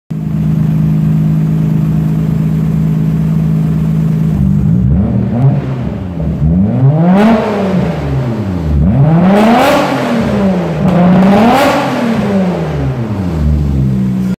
Nissan Skyline r32 GTR - Sound check equal length downpipe
Sound check after replace of the exhaust with equal length downpipe.